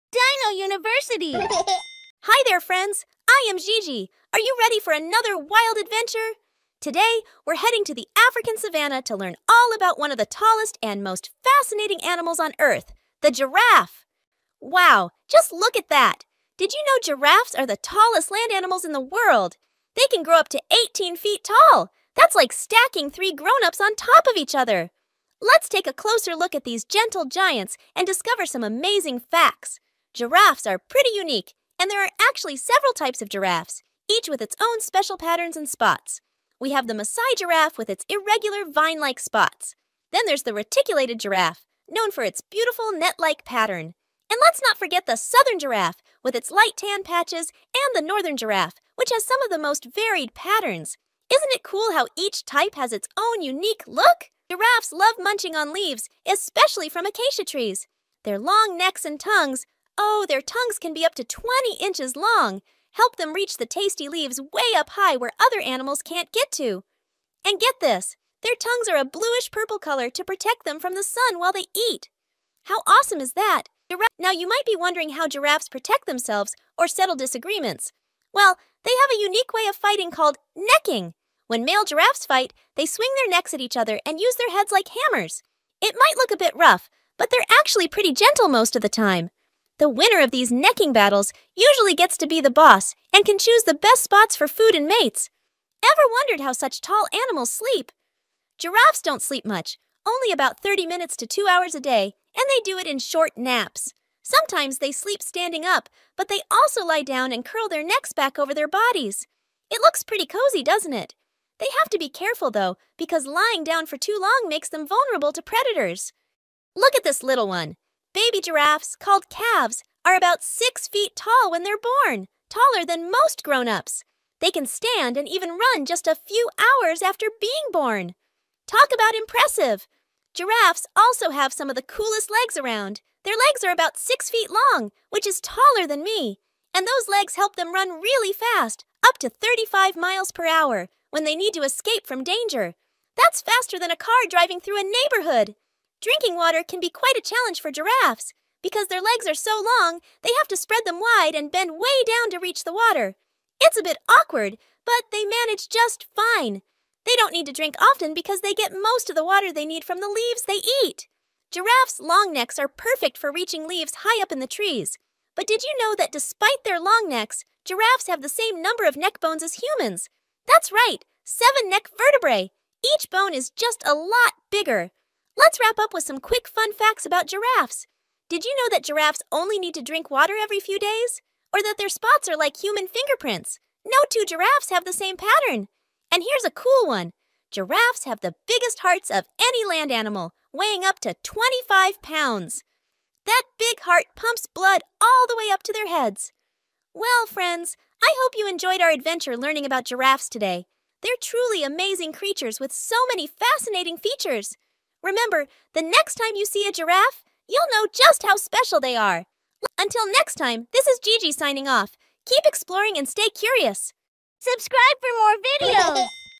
Giraffes Fight with Necks? Here's Why!
Kid friendly videos explaining about various animals and wildlife